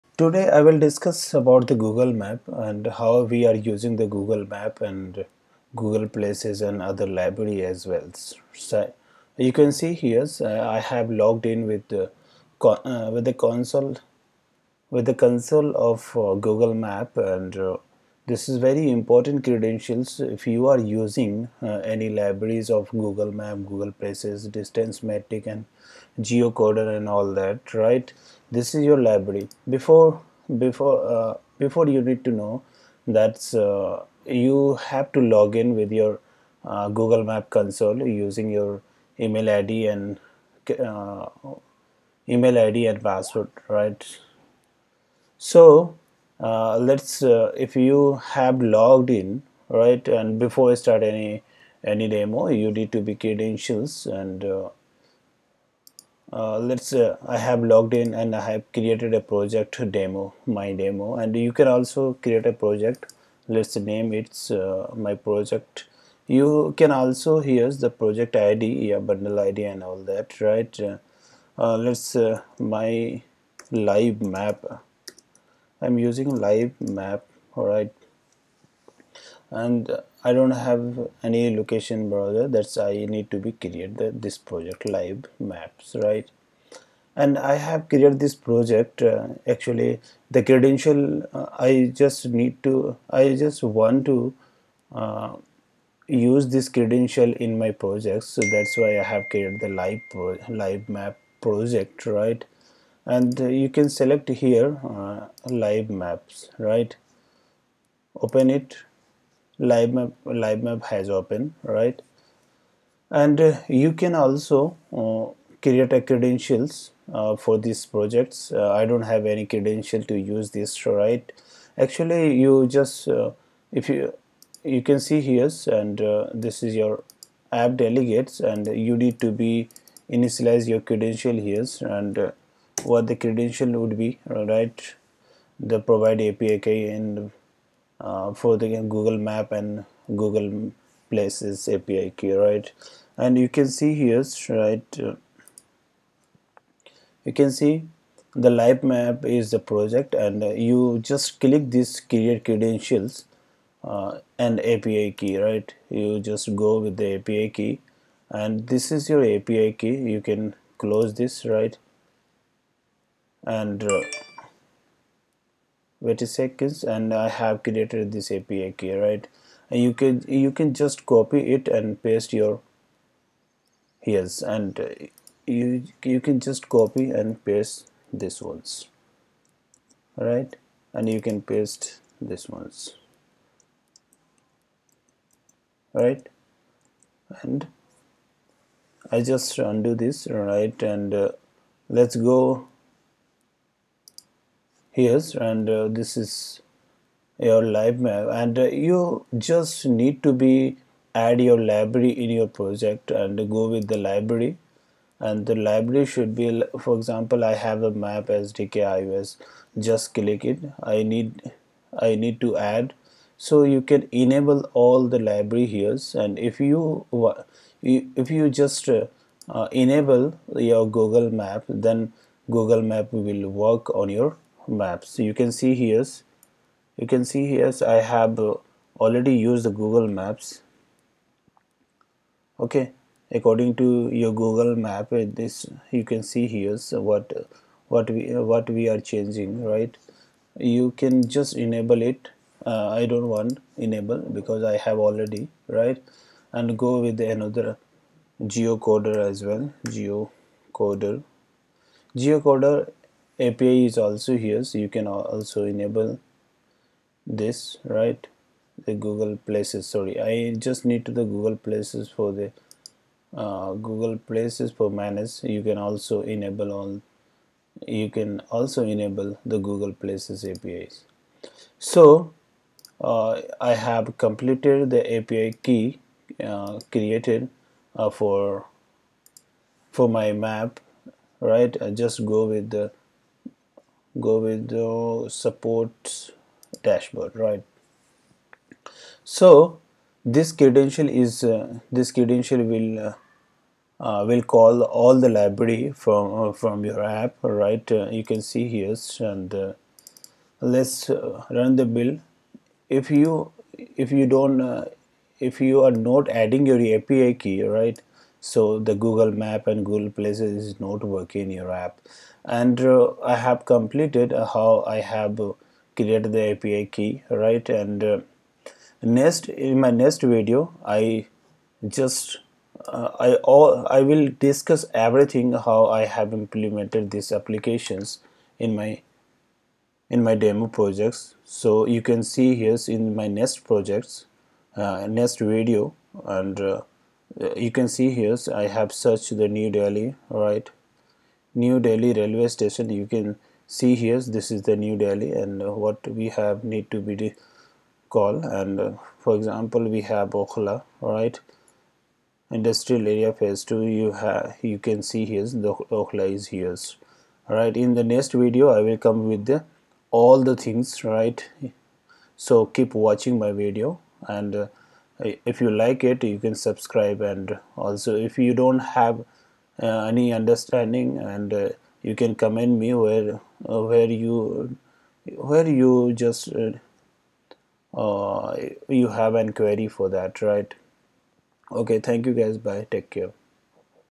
google map Implementation in swift part-1
In this video you can learn how can we create a project in google map console for and create api key and manage the library.